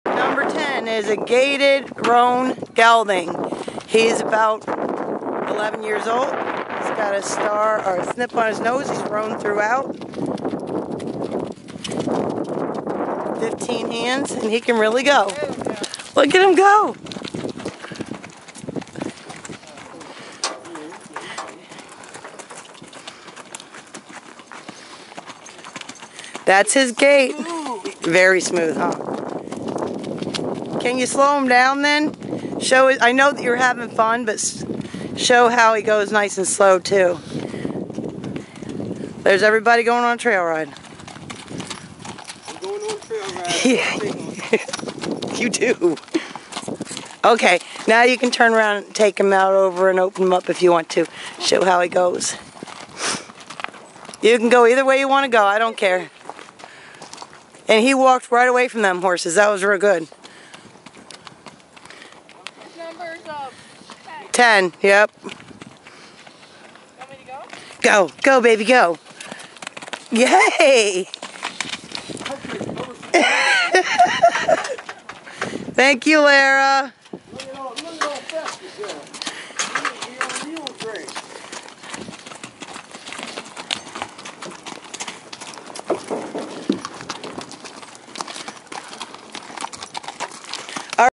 1-30-10 - Bay roan gaited gelding VIDEO 1
1-30-10 - Bay roan gaited gelding approximately 15 hands rode walk, trot, and canter with a halter and lead rope. The auctioneer stated that he was owned by a local family and it was their kids horse. He also stated that he was rode on trails.

$485.00